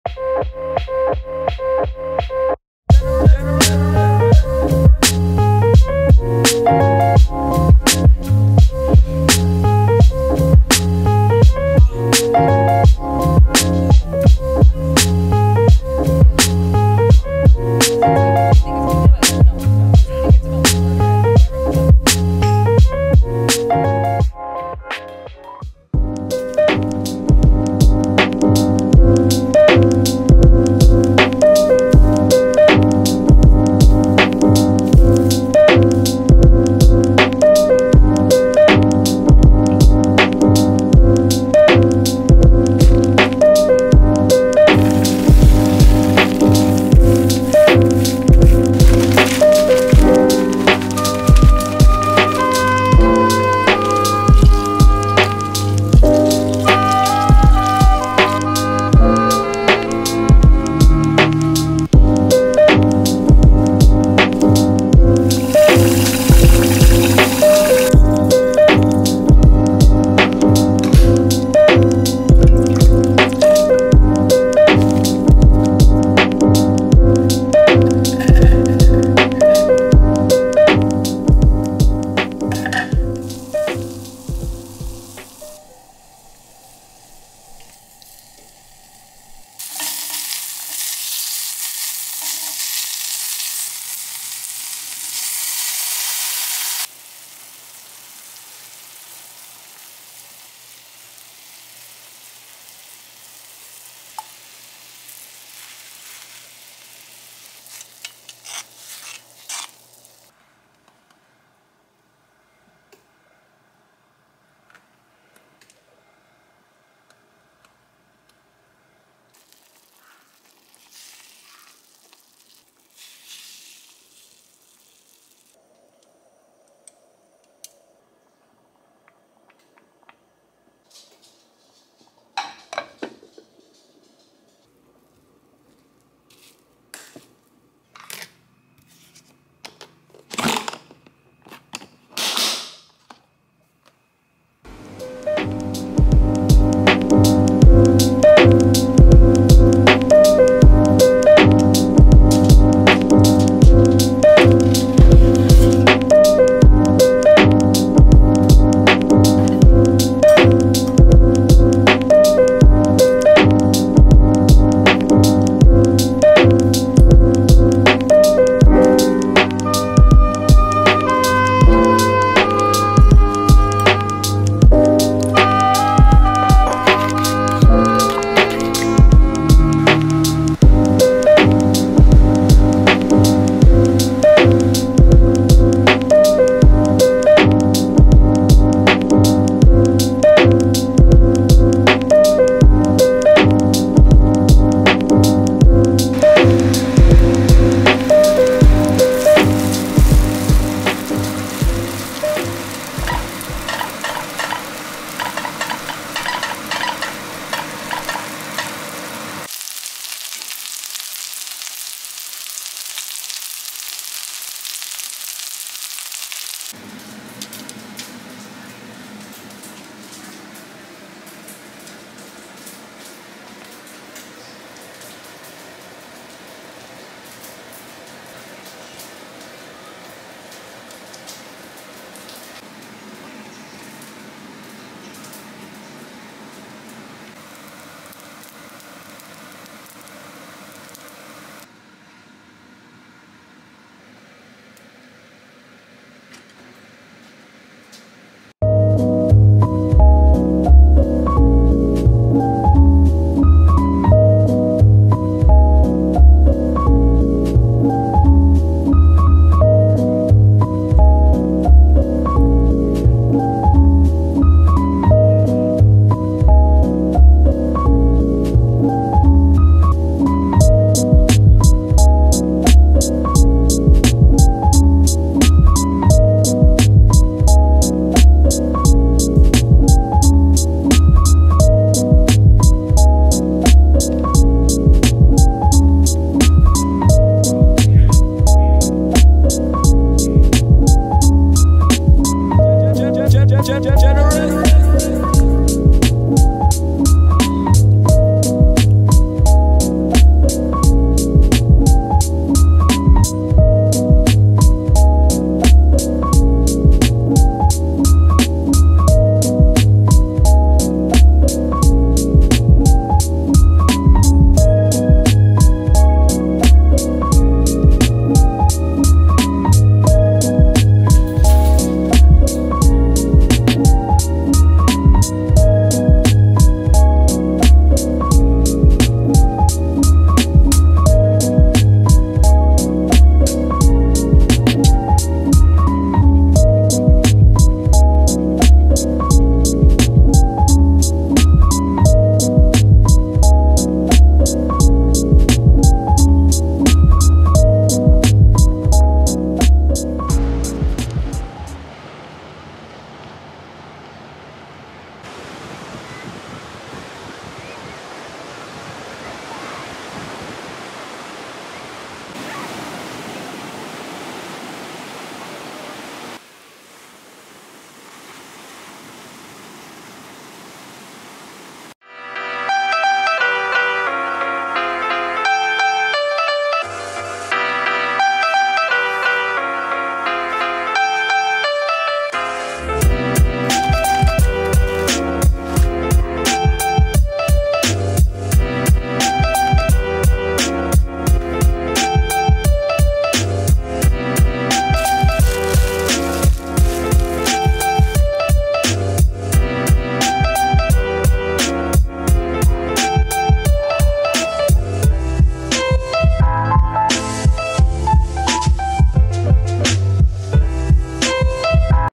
[Vlog] LA diaries ☀️ | Ktown, San Diego trip, spicy salmon and crispy rice | ロサンゼルスの暮らし
Going to San Diego is always calming for me. It's like going on a mini vacation 🌴☀️

. . . foodstagram . . .

. . . f i l m i n g  &  e d i t i n g . . .